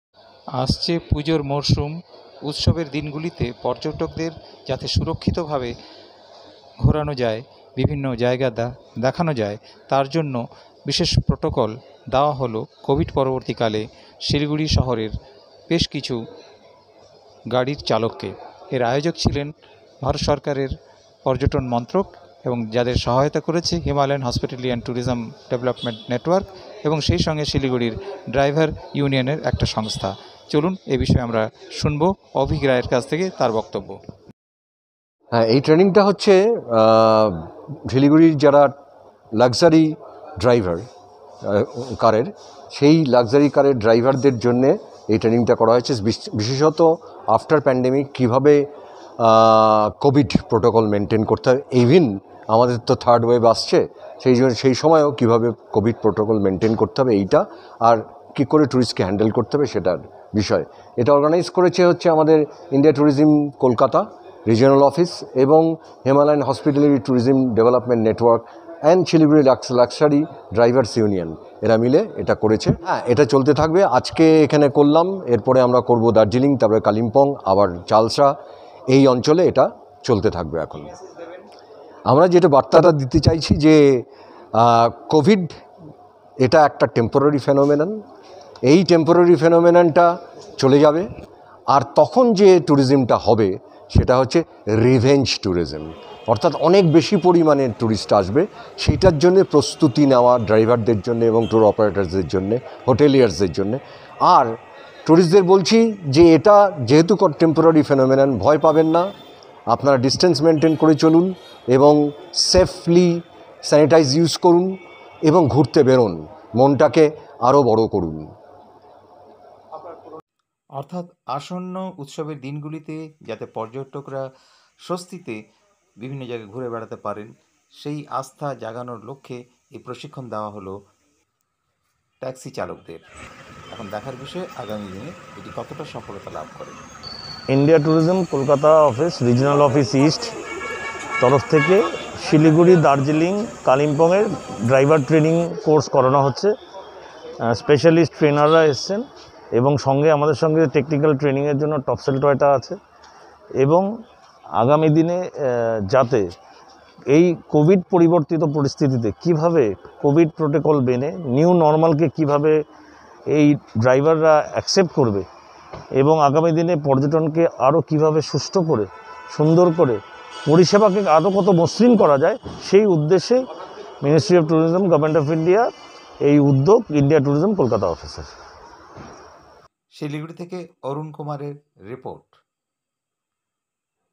পুজোর মরসুমে পর্যটকদের পরিবহন চালকদের কোভিড প্রটোকল বিশেষ প্রশিক্ষণ। খবরের দুনিয়া।।
পুজোর মরসুমে পর্যটকদের পরিবহন চালকদের কোভিড প্রটোকল বিশেষ প্রশিক্ষণ

 অরুণ কুমার:শিলিগুড়ি
আসন্ন পুজোর দিন গুলিতে উত্তরবঙ্গে বিশেষ করে দার্জিলিং জলপাইগুড়ি ডুয়ার্স প্রভৃতি এলাকায় পর্যটকদের আসার সম্ভাবনা কথা মাথায় রেখে এই অঞ্চলে আরম্ভ হয়েছে পর্যটকদের নিয়ে যারা আসা-যাওয়া করেন সেই সমস্ত ট্যাক্সিচালকদের বিশেষ প্রশিক্ষণ দেয়া আরম্ভ করেছে ইন্ডিয়া ট্যুরিজম সহ পর্যটন শিল্পের সঙ্গে যুক্ত বিভিন্ন সংস্থা প্রতিষ্ঠান। শিলিগুড়িতে আরম্ভ হয়েছে এ প্রশিক্ষণ যেখানে প্রাথমিক পর্বে প্রায় ত্রিশ জনকে কোভিড পরবর্তী সময়ে কিভাবে পর্যটকদের তারা বিভিন্ন জায়গায় ঘোরাতে নিয়ে যাবেন কি কি সতর্কতামূলক ব্যবস্থা গ্রহণ করবেন সে বিষয়ে যাবতীয় প্রটোকল প্রশিক্ষণ দেওয়ার কাজ শুরু করেছে। শিলিগুড়ি শহরের একটি বেসরকারি হোটেলে প্রথমে প্রাথমিক প্রটোকল গাইডলাইন প্রশিক্ষণ দেওয়ার পাশাপাশি গাড়িচালকদের হাতে কলমে শেখানো হয়েছে কিভাবে তারা পর্যটকদেরকে আসার পর বিভিন্ন জায়গায় ঘোরানো এবং তাকে গন্তব্যস্থলে পৌঁছে দেওয়া সময় কিভাবে তাদের পর্যটনের আনন্দ সুরক্ষিতভাবে প্রদান করা হবে। এ বিষয়ে ইন্ডিয়া ট্যুরিজমের বিশেষ প্রশিক্ষক শমিক রায় জানিয়েছেন, পর্যটন মরসুমে নির্ভয়ে কোভিড প্রটোকল মেনে বেড়াতে আসুন পর্যটকরা, আমরা প্রস্তুত পর্যটকদের সুরক্ষা প্রদান করার পাশাপাশি পর্যটনের দিনগুলি যাতে আনন্দে কাটাতে পারেন সেদিকে লক্ষ্য রেখে আমরা শিলিগুড়ি তথা তার আশপাশের প্রায় 30 জন গাড়িচালককে এ প্রশিক্ষণ দিয়েছি এবং এটা চলতে থাকবে। উল্লেখ করা যেতে পারে যে ধীরে ধীরে মহামারীর প্রকোপ হ্রাস পেতে থাকায়  ও সেইসঙ্গে বনাঞ্চল গুলি পর্যটকদের জন্য খুলে দেওয়ার নির্দেশ জারি হওয়ার সঙ্গে সঙ্গে পর্যটকদের আগমন শুরু হয়ে গিয়েছে উত্তরবঙ্গে। সেই বিষয়টি গুরুত্ব দিয়ে বিবেচনা করে এই অঞ্চলের যারা পর্যটন শিল্পের সঙ্গে যুক্ত সংস্থা তারা উদ্যোগী হয়েছেন কোভিড সংক্রান্ত প্রটোকল কার্যকরী করার ক্ষেত্রে। উত্তরবঙ্গের পর্যটন বিশেষজ্ঞ সম্রাট সান্যাল জানিয়েছেন ইন্ডিয়া ট্যুরিজম ও শিলিগুড়ির পর্যটক সংস্থা মিলিত ভাবে এই কাজ শুরু করেছে যাতে পর্যটকদের বিভিন্ন জায়গায় বেড়াতে গিয়ে কোনো রকম সমস্যায় পড়তে না হয়। কোভিড প্রটোকল এর যাবতীয় নির্দেশিকা তারা পালন করার ক্ষেত্রে সচেষ্ট থাকবেন আগামী উৎসবের দিন গুলিতে। আগামী দিনে শিলিগুড়ি ছাড়াও দার্জিলিং কার্শিয়াং কালিম্পং এবং ডুয়ার্সের বিভিন্ন গাড়িচালকদের এই কোভিড প্রটোকল সংক্রান্ত প্রশিক্ষণ এর কাজ দ্রুত শেষ করা হবে বলে ইন্ডিয়া ট্যুরিজমের বিশেষ প্রশিক্ষক শমিক রায় জানিয়েছেন।